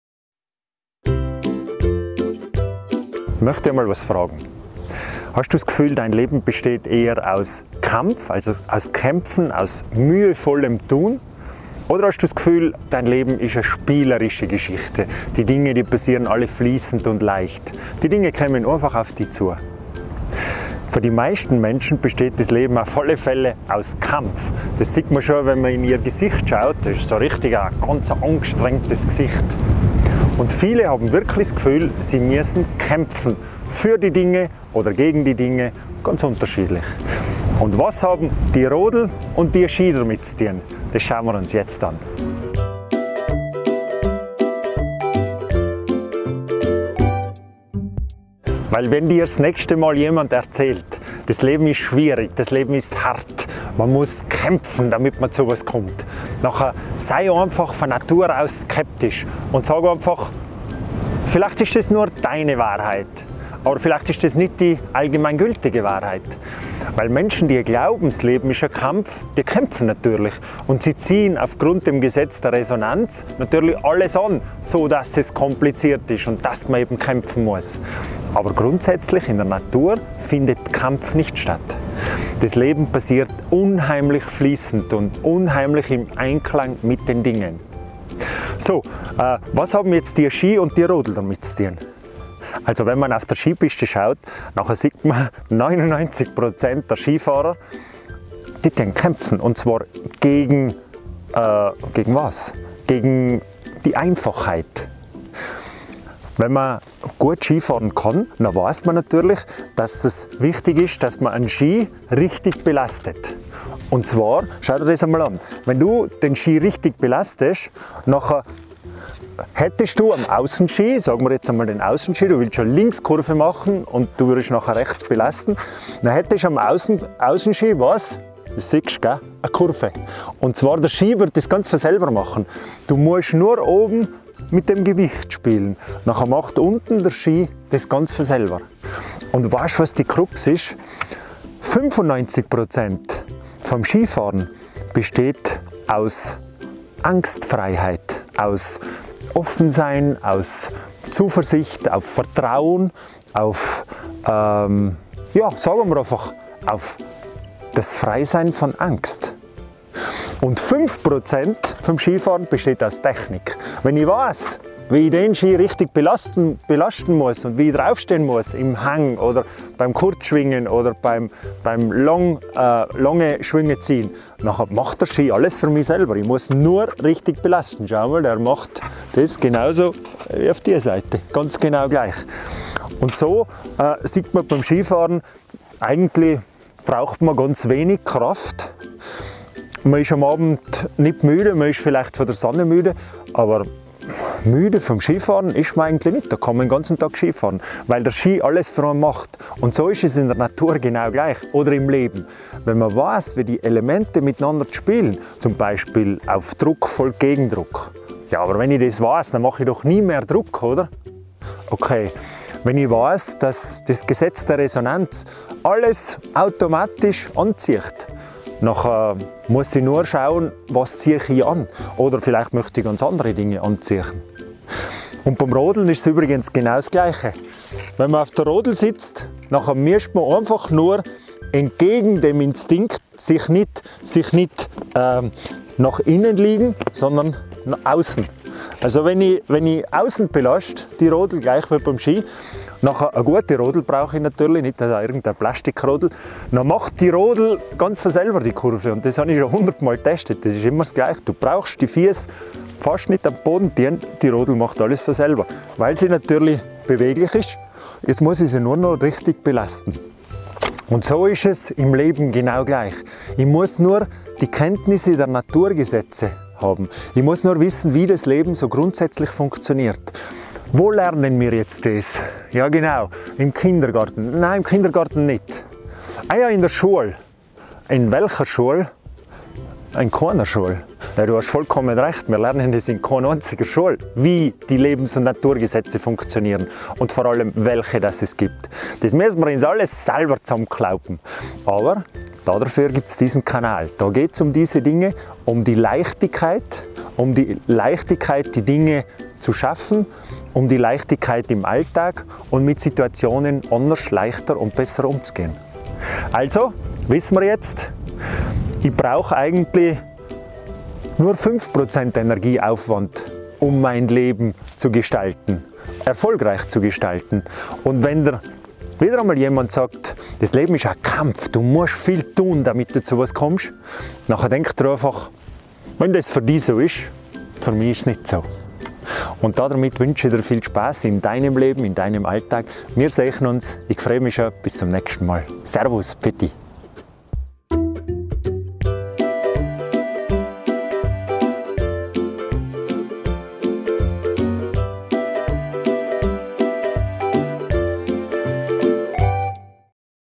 Das Leben ist einfach - kein Kampf? So wird dein Leben ein Spiel!
Lerne dein Leben zu spielen, anstatt zu kämpfen. So entsteht neue Energie und neue Kraft für dich, die durch dich strömt.
Ein Lebensstrom, der dich fördert und dir hilft, der dir alles bringt, was dein Leben angenehm und erfolgreich macht.